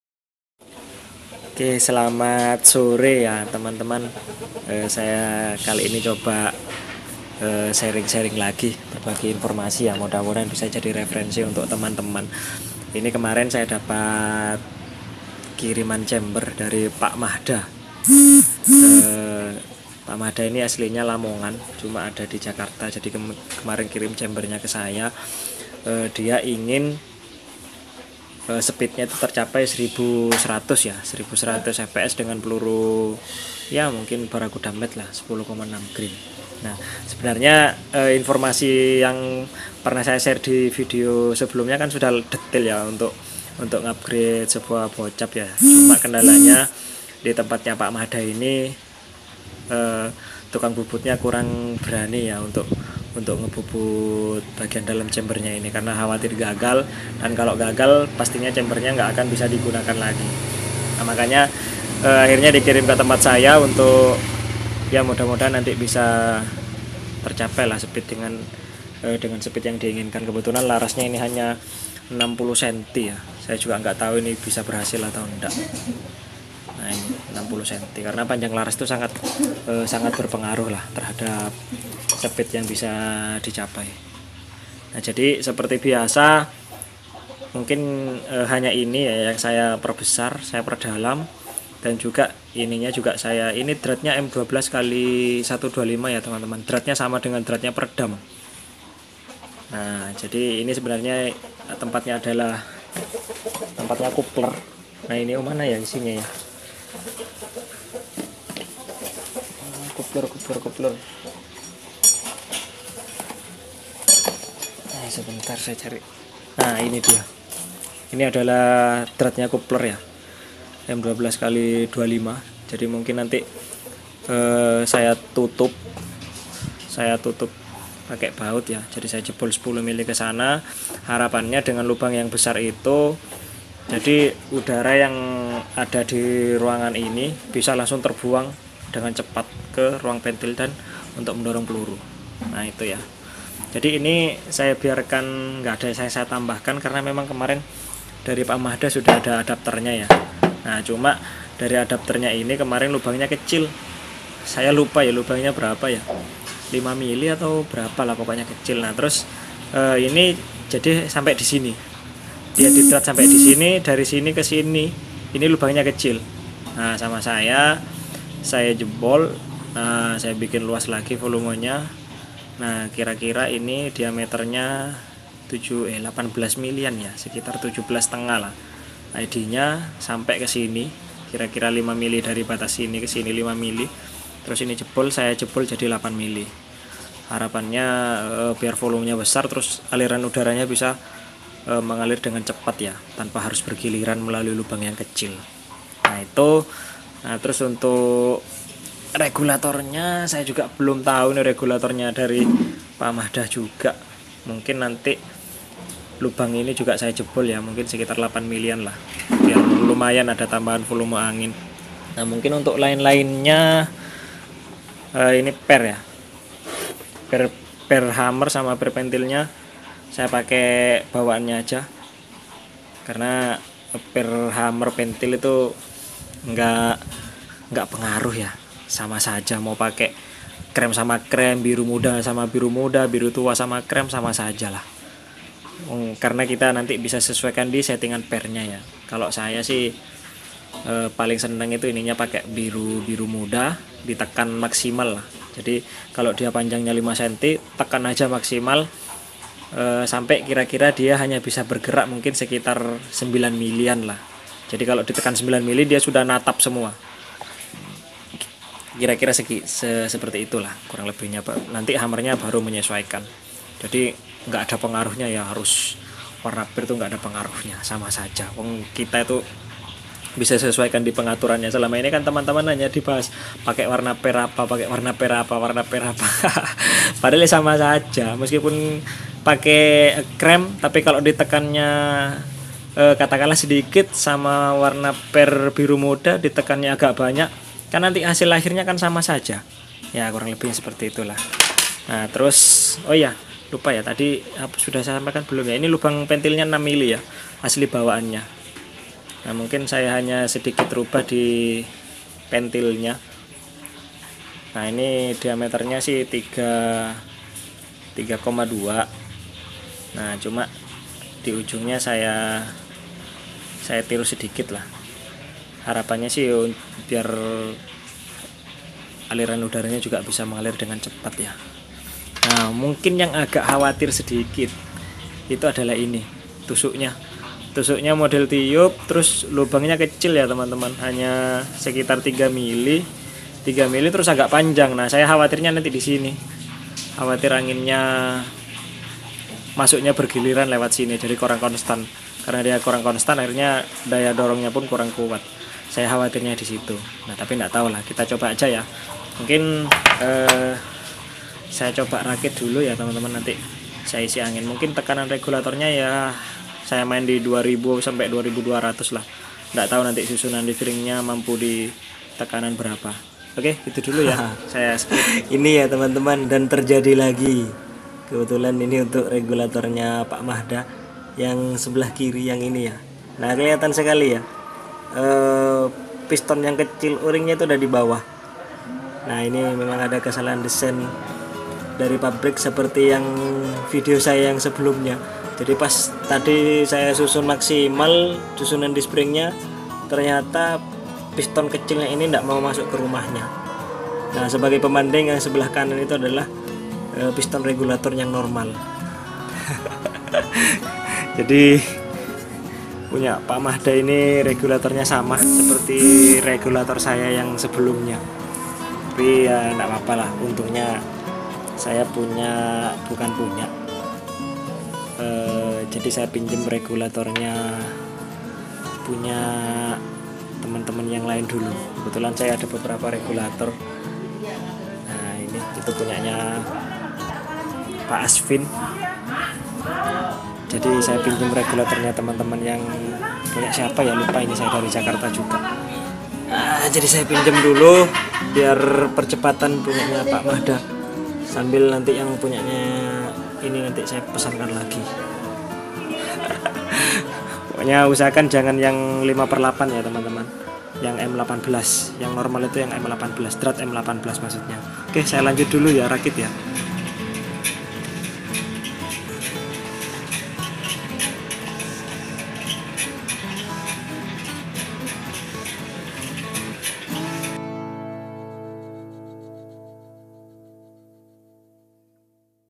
Upgrade Chamber Bocap milik pak Mahda Prt 1/3
Halo.. teman-teman bediler di dunia maya
kali ini saya coba berikan informasi ataupun sharing pengalaman dari riset atau percobaan yang saya lakukan, kebetulan skrg ini lagi mencoba setting chamber milik kawan youtube dari Jakarta
berharap bisa speed diatas 1100 fps dengan mimis 10,6 grain pada tekanan regulator sekitar 2200 psi.

untuk hasil juga masih belum tahu seperti apa...
mohon koreksi dan masukannya jika ada yang kurang atau ada metode yang lebih baik dalam mencapai speed yang diinginkan.